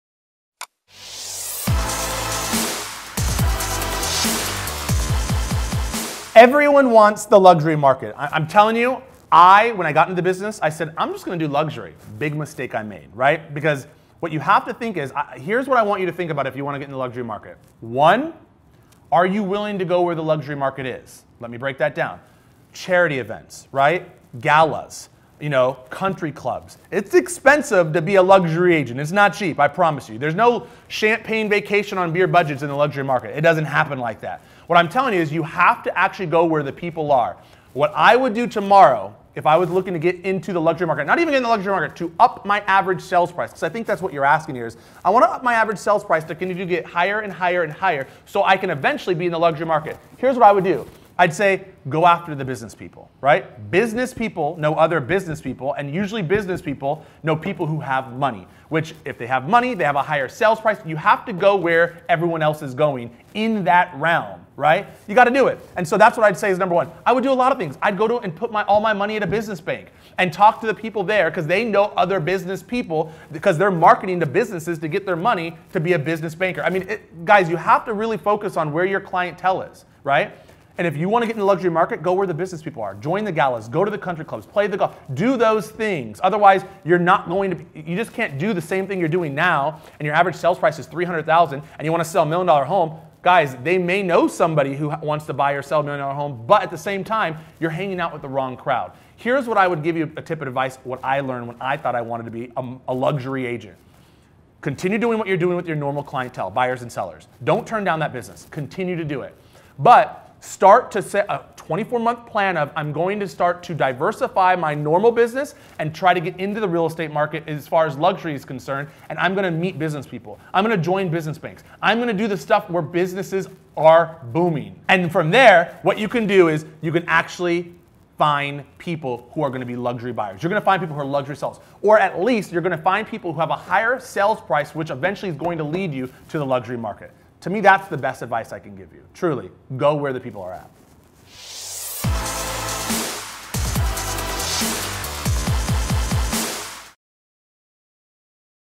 What It Takes to Be Successful in the Luxury Real Estate Market
💰💰  The million dollar question for million dollar sales 💰💰

Everyone wants to get into the luxury market but often times you don't realize what it takes to really be successful in the luxury space.

Are you willing to go where the lux market is?
What I mean is this market requires attending charity events, galas, country clubs, etc. It's not cheap to participate in these functions.

"There's no champagne vacations on a beer budget in the luxury market."

What I think you're actually asking is how do you up your average sales price. And here's what I would do if I wanted to increase my average sales price.